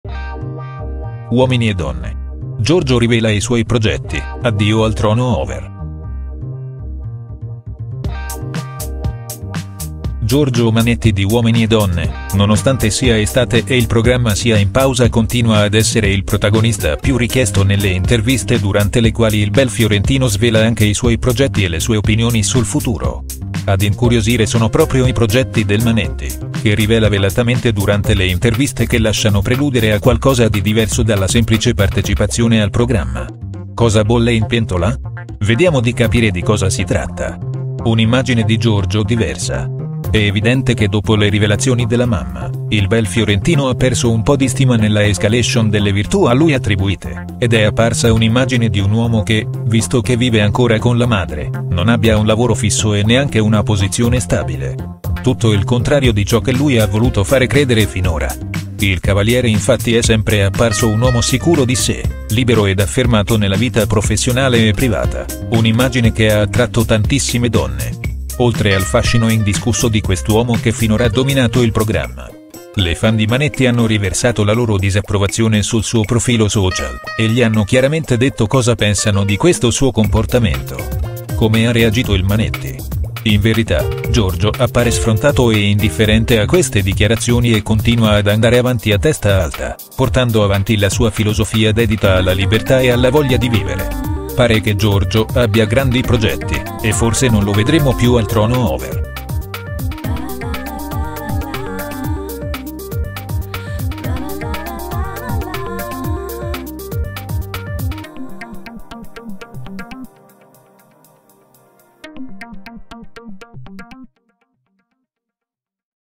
Uomini e Donne. Giorgio Manetti rivela i suoi progetti, addio al trono over
Nonostante Uomini e Donne sia in ferie Giorgio Manetti, l'affascinante cavaliere del trono over, continua a far parlare di se. Secondo alcune indiscrezioni sembra che la sua partecipazione alla nuova stagione del trono over non sia poi così scontata.